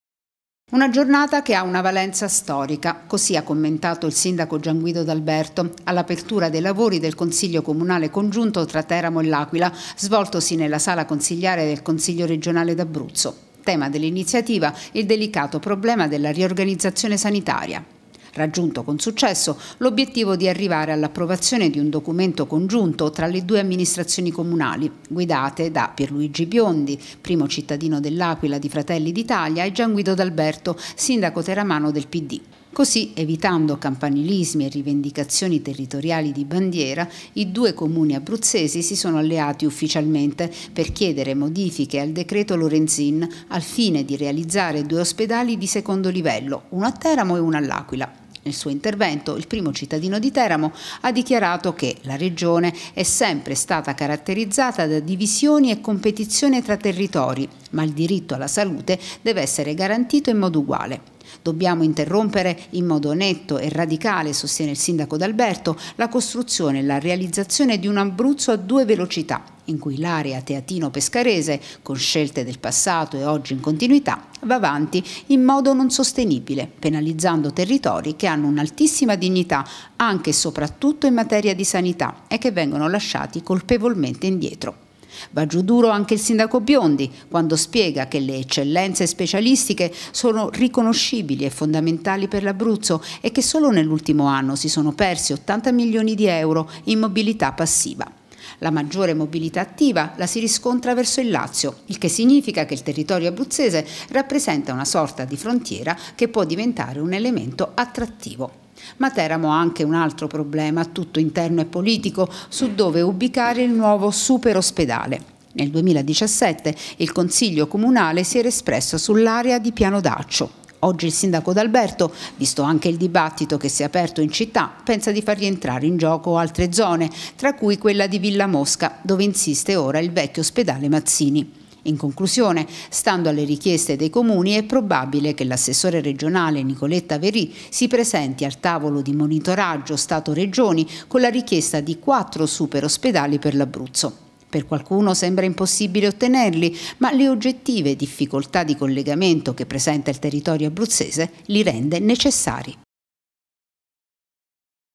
Super ospedali: Teramo e L'Aquila ne reclamano uno ciascuno
Si è concluso con la firma di un documento unico il consiglio comunale congiunto tra Teramo e L'Aquila sul delicato tema della riorganizzazione sanitaria. Le due amministrazioni hanno chiesto modifiche al decreto Lorenzin per realizzare due ospedali di II livello, uno nel comune teramano e uno in quello aquilano. La parola passa ora alla Regione Abruzzo.